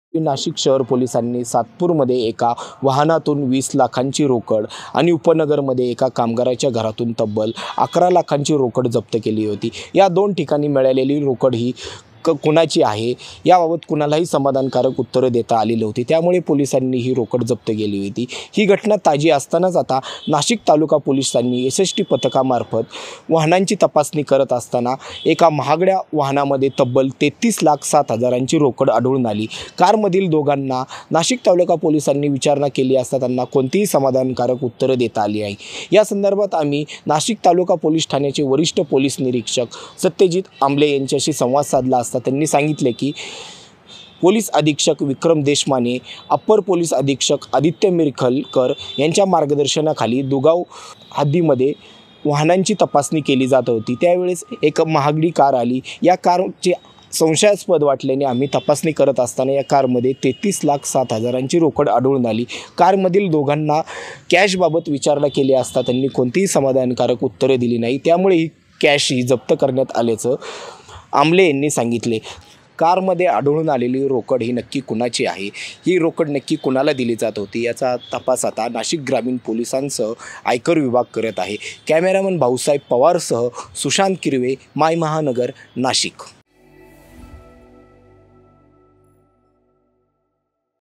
Nashik : महागड्या कारमधून 33 लाखांची रोकड जप्त, नाशिक ग्रामीण पोलिसांची मोठी कारवाई
विधानसभा निवडणुकीसाठी प्रचार सुरु झाला असून त्याच धामधुमीत बेकायदेशिरपणे ३३  लाख ७ रुपयांची बेहिशेबी रक्कम कारमधून नेणार्‍या दोघांना नाशिक तालुका पोलिसांनी ताब्यात घेतले आहे. नाशिक तालुक्यातील दुगाव येथे बुधवारी (दि. ६) रात्री ही कारवाई करण्यात आली असून, पोलिसांनी आयकर विभागासह एसएसटी पथकास कळविले आहे.

EPK92|AA